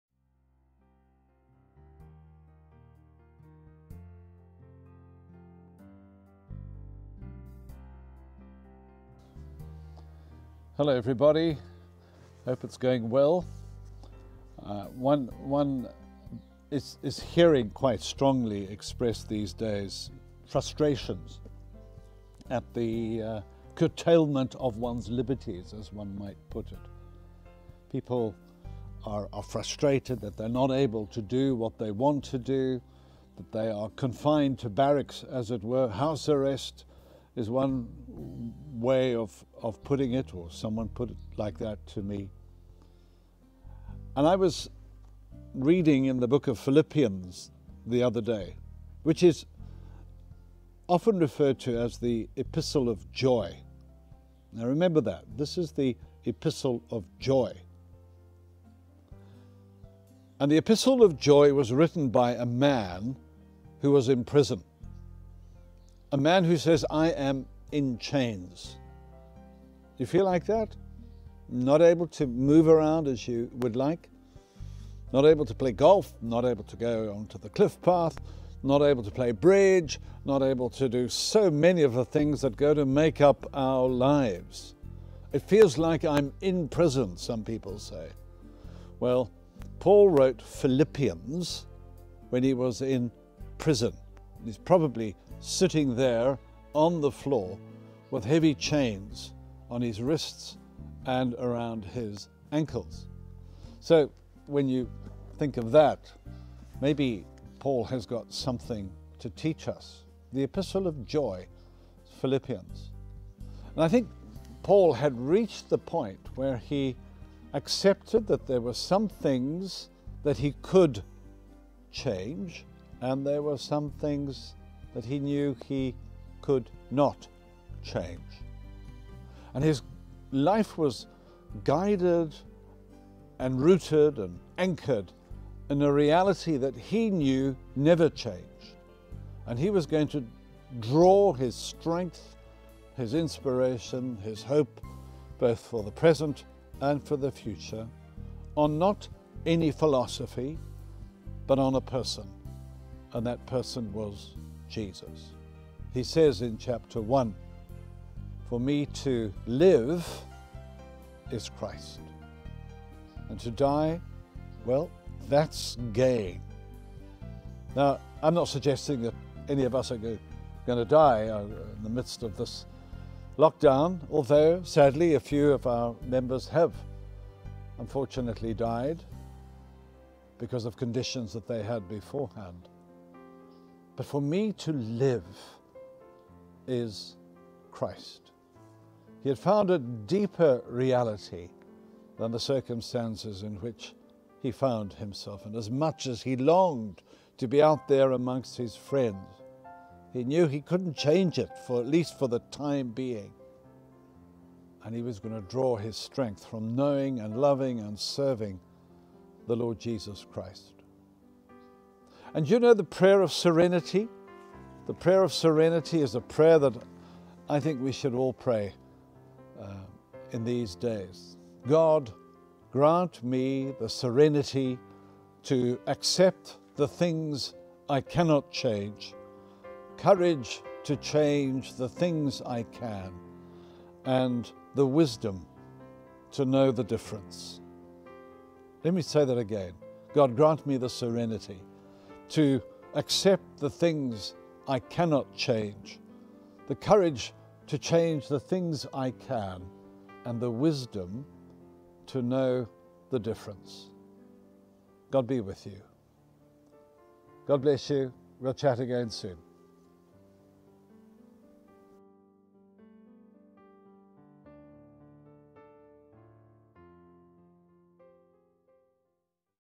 Garden Reflection#2
Devotion after lockdown level 4 announcement.
5 weeks and still some way to go.
We may find it difficult to deal with the restrictions, keep focused upward with faith and hope.